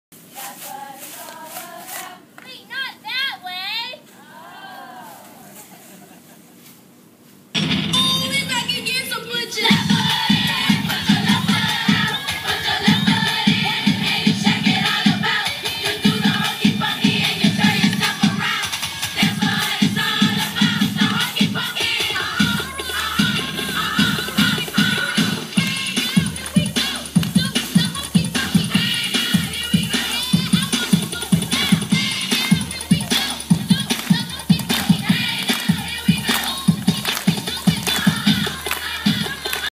Project Fit America 4
CHS Cheerleaders perform at the Project Fit America Dedication